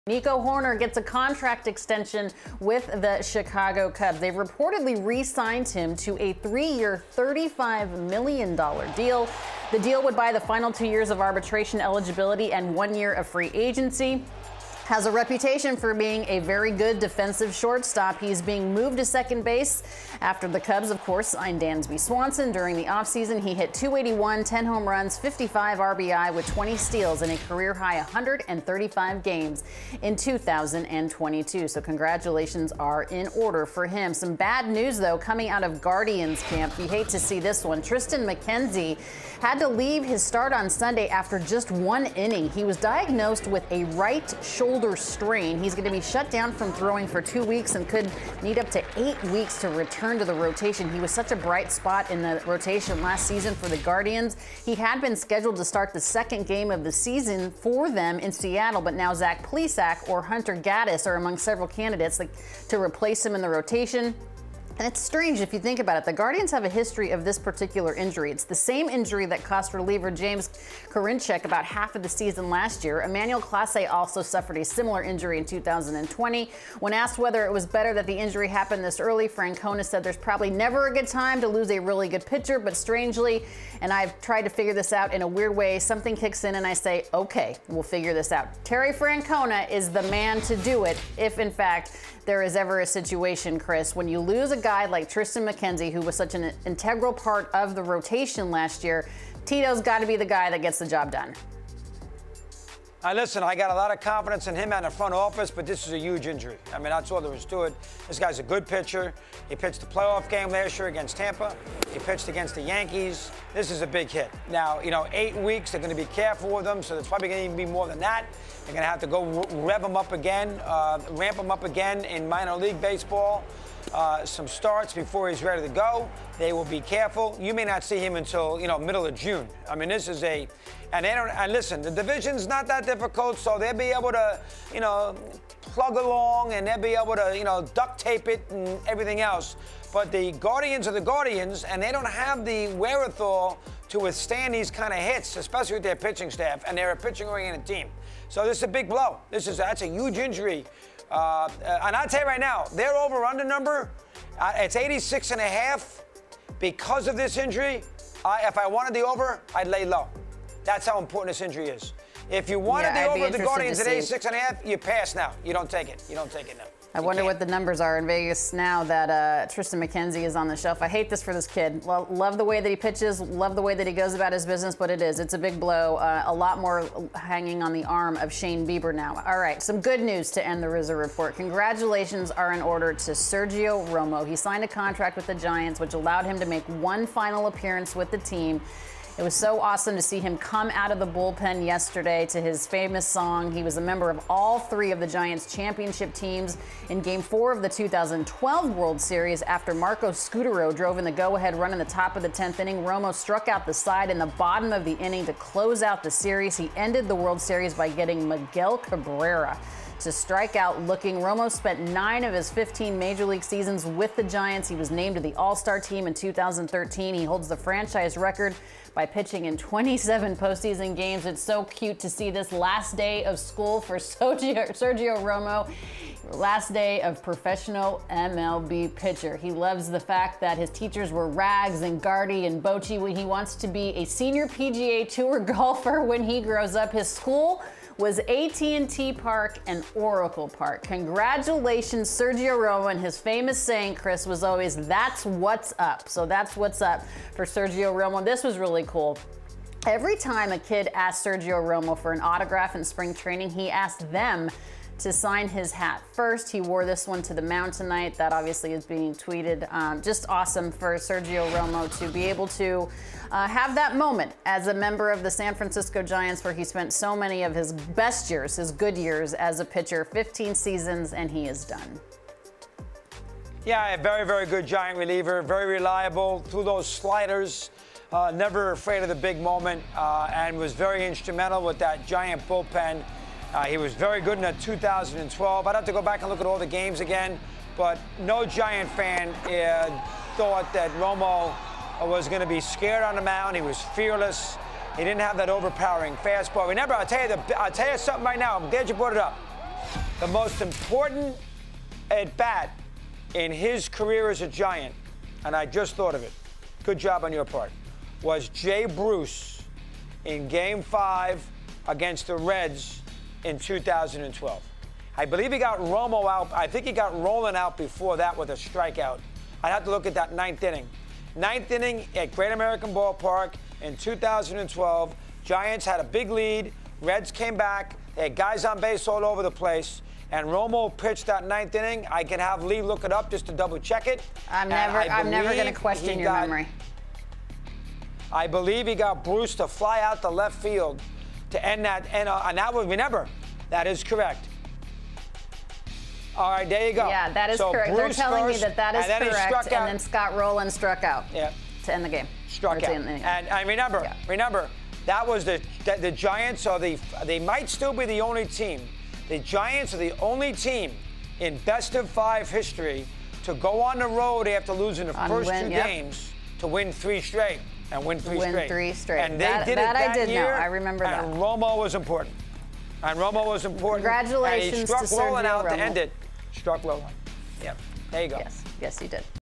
Rizzo Report on Hoerner and more
Alanna Rizzo discusses the extension for Nico Hoerner, the injury to Triston McKenzie and the send off the Giants gave to Sergio Romo